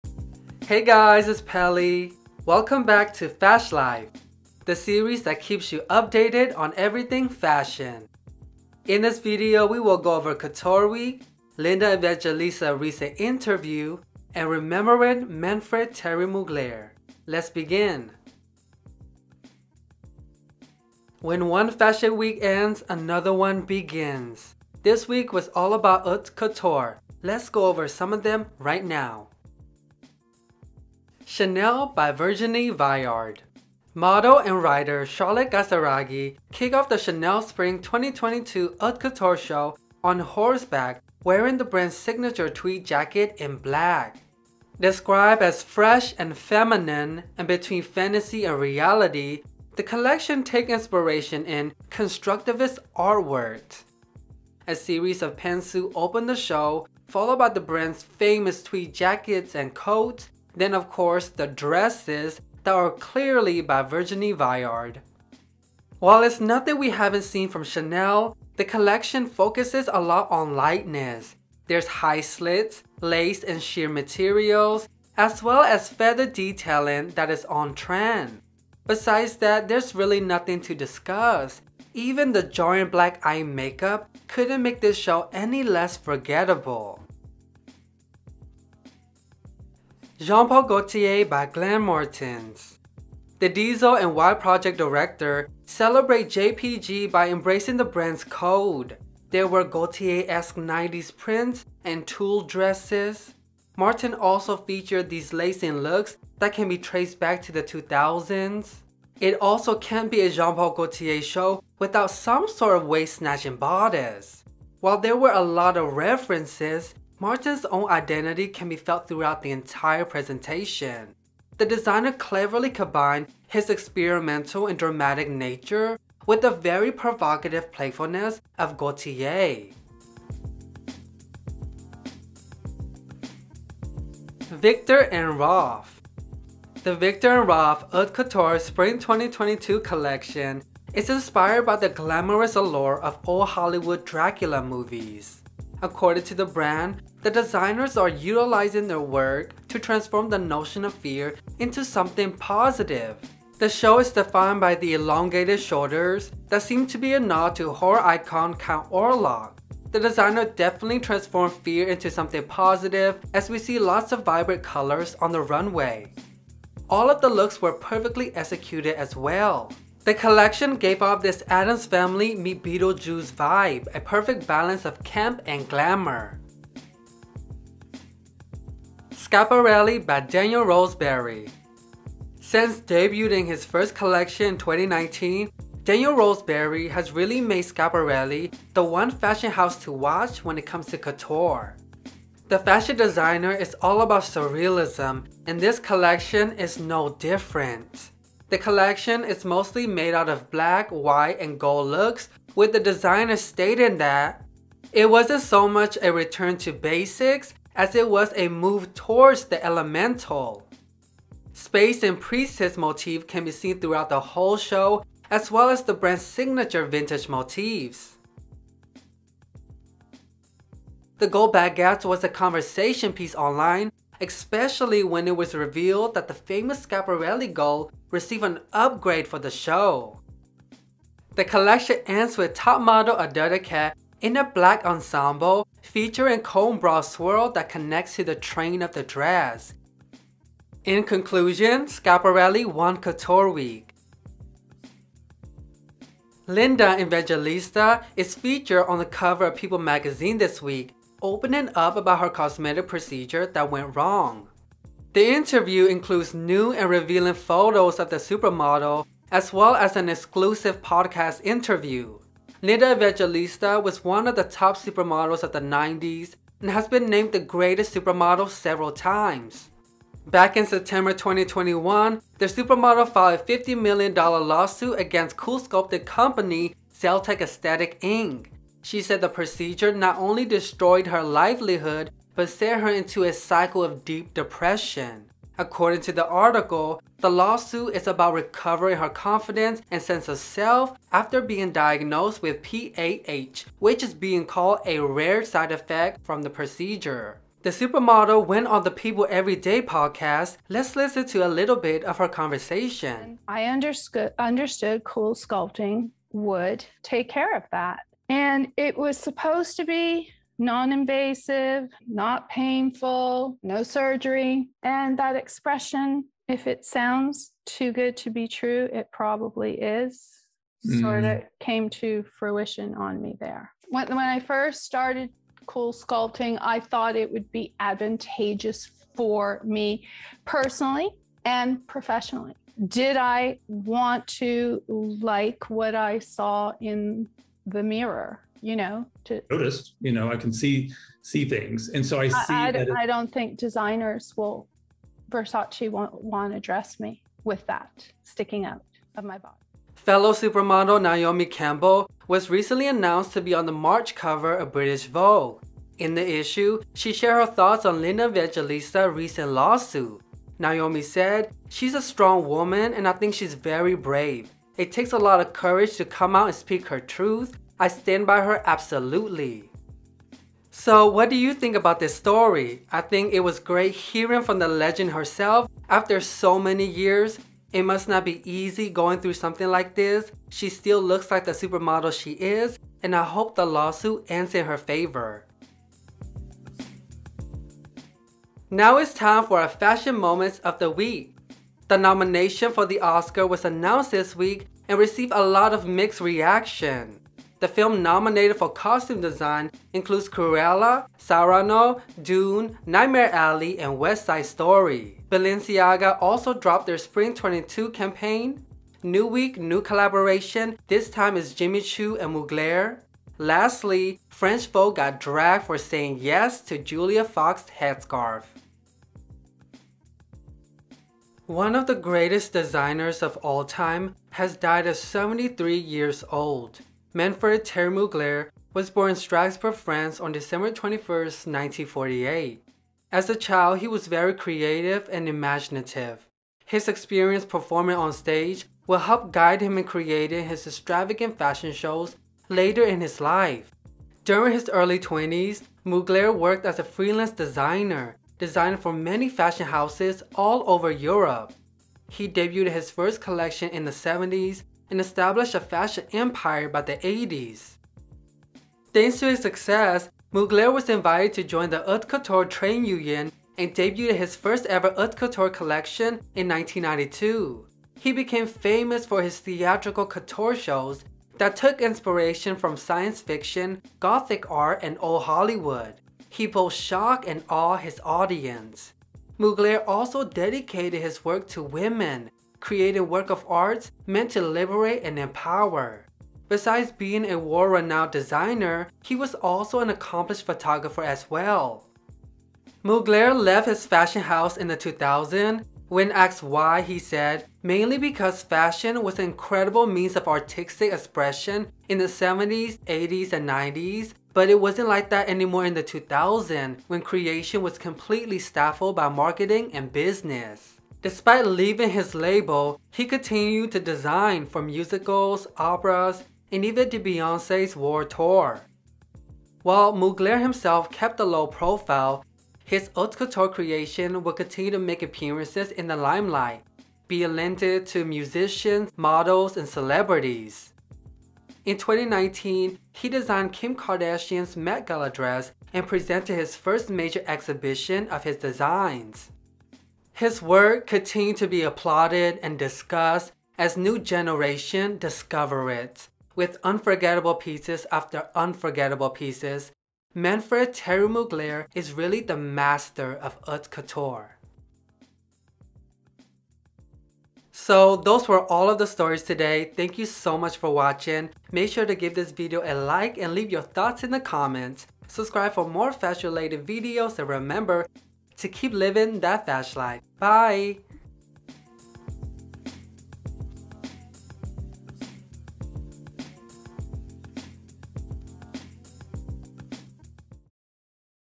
Fashion of the Week: Remembering Thierry Mugler, Linda Evangelista Interview & Spring 2022 Couture
Hey Fashionista!

Welcome back to Fash Life,
The series that keeps you updated on everything fashion.
In this video we will go over Couture week, Linda Evangelista recent Interview and remembering Manfred Thierry Mugler.

Timestamp:

Intro: 0:00

Haute Couture Week: 0:20

Linda Evangelista & People Magazine: 4:06

Moments: 6:28

Remembering Manfred Thierry Mugler: 7:01

Outro: 9:17